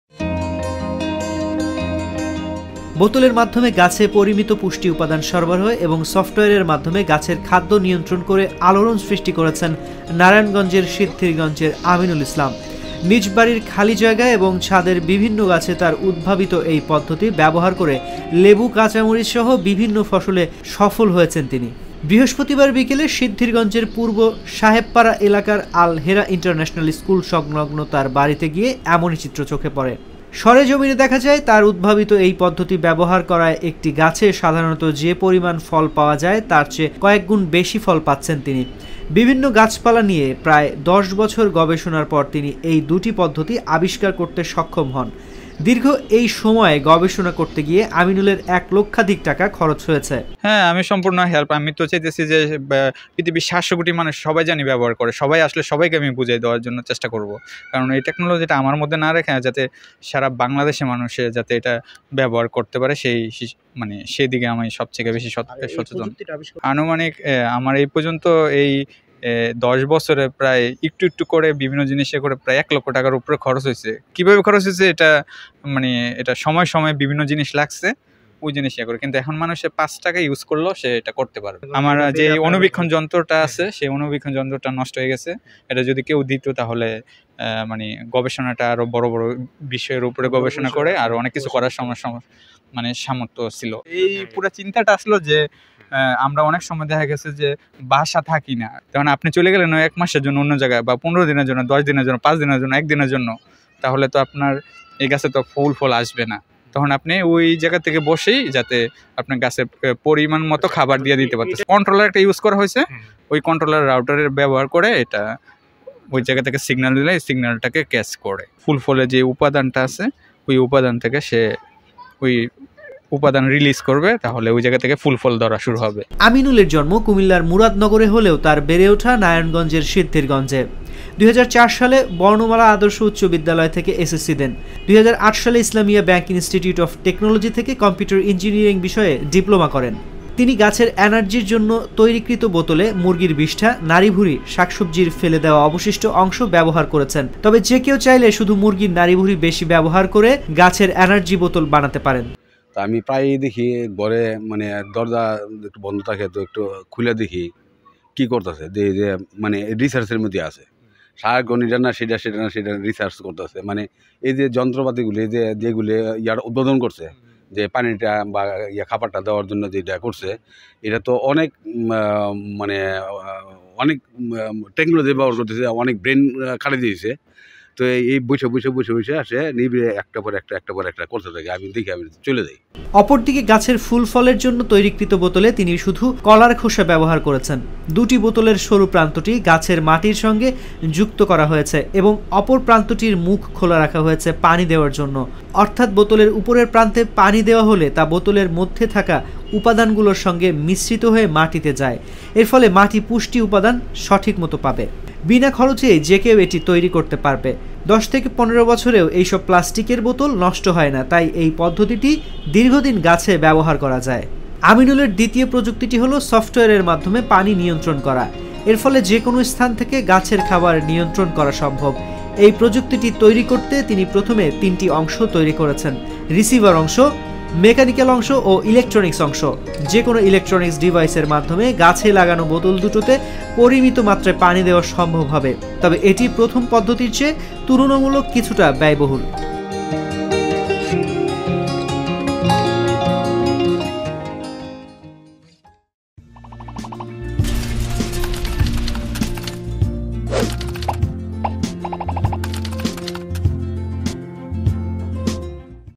বোতল ও সফটওয়্যারে গাছের খাদ্য নিয়ন্ত্রণ করেন আমিনুল || Jago News
বোতল ও সফটওয়্যারে গাছের খাদ্য নিয়ন্ত্রণ করেন আমিনুল